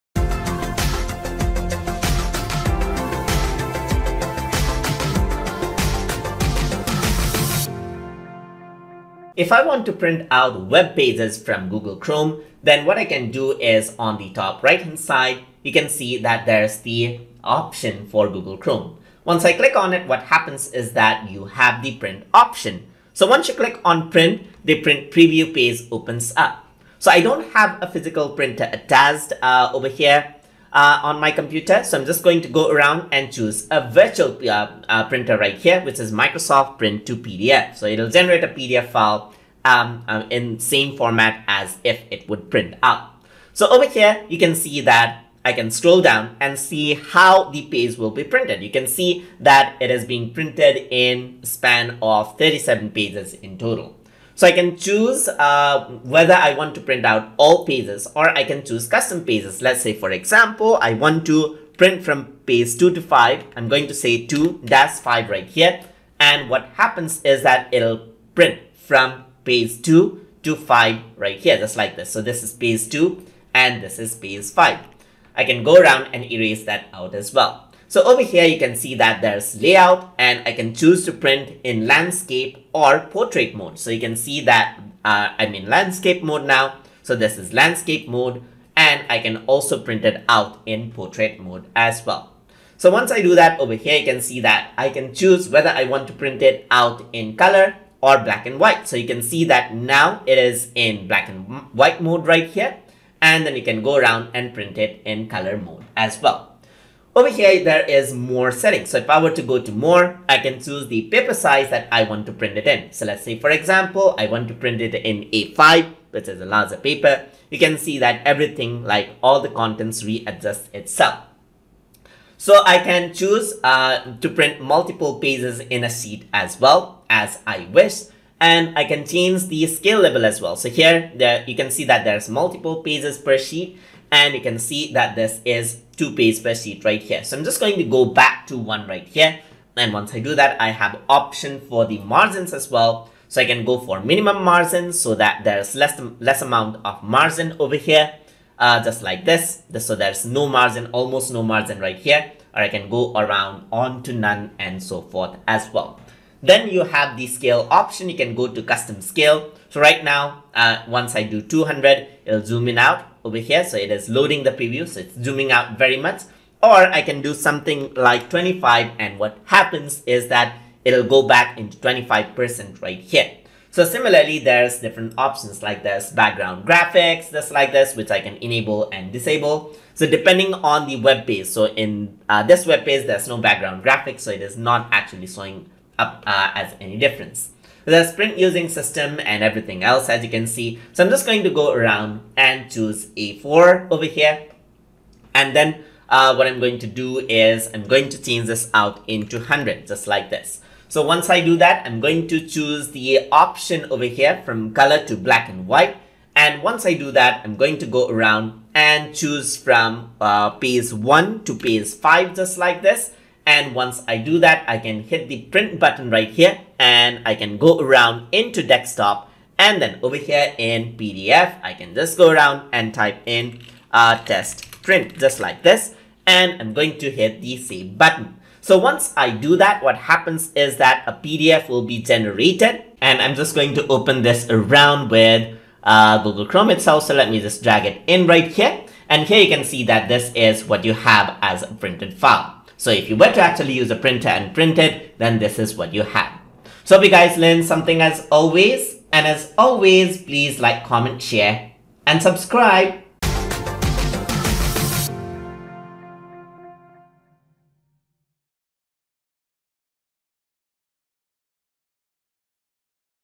Google Chrome - Tutorial 24 - Printing Web Pages
In this tutorial, we will be discussing about Printing Web Pages in Google Chrome

Learn the basics of using Google Chrome including browse, manage and work with extensions.

This lesson has been made using Google Chrome for Windows but is applicable for App Version and Mac Version of Google Chrome Application as well.